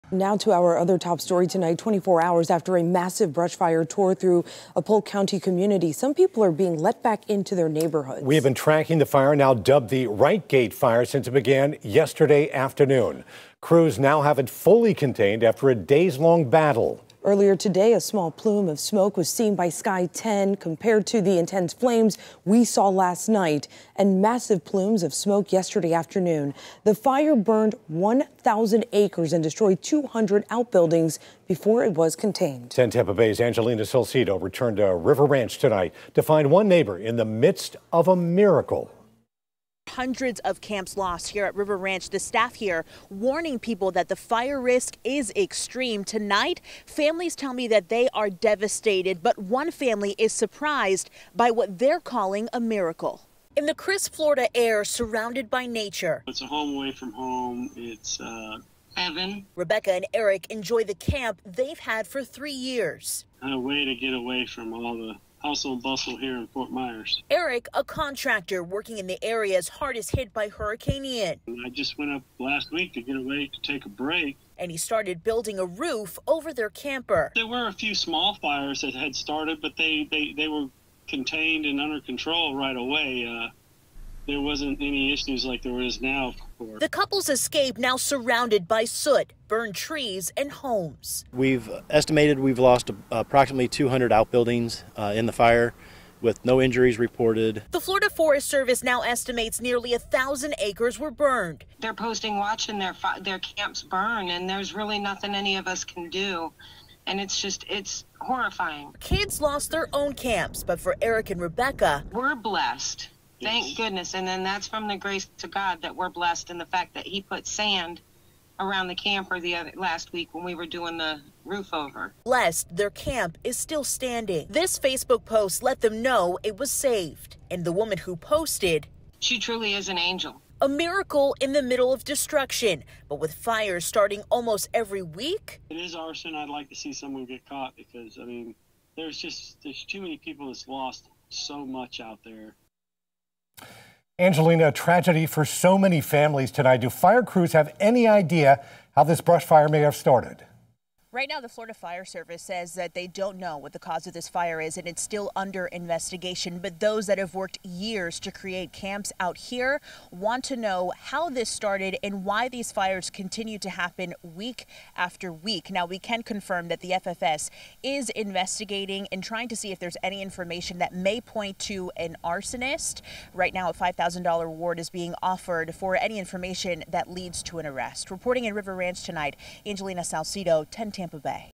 People let back into Polk County neighborhoods after massive brush fire
According to the Florida Forest Service, a brush fire in the River Ranch area of Polk County dubbed the Right Gate fire, is now 100% contained.

The brush fire was first reported Tuesday afternoon and is located off State Road 60, just east of Frostproof. Evacuations were reportedly underway Tuesday for homes and buildings in the Canopy Oaks portion of River Ranch.

Approximately 200 outbuildings have been lost as a result of the blaze.